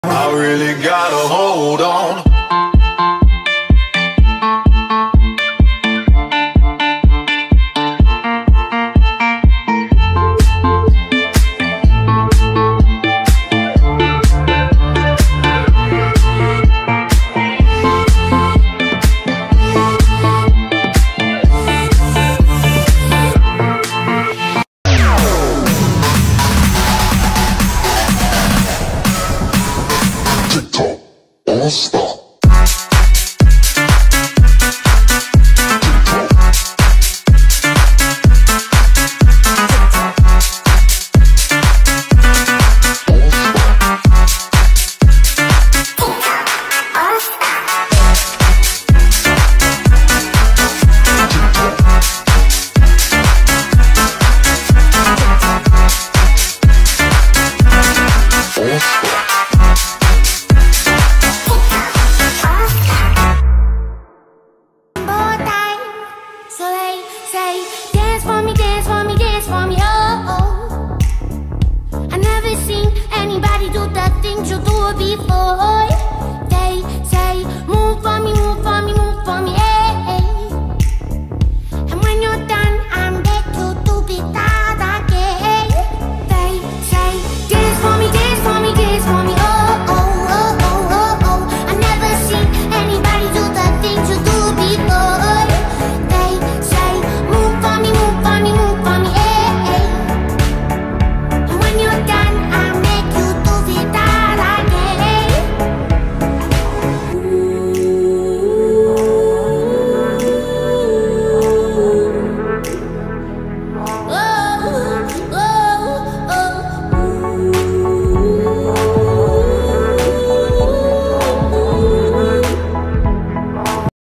CALISTHENICS HOTBOY DANCE ON PULL-UP BAR | Trai Đẹp Nhảy Trên Xà Đơn Street Workout | Lê Phạm Thế Vũ
Calisthenics Handstand Artist - Le Pham The Vu - 23 years old - VIETNAM 🇻🇳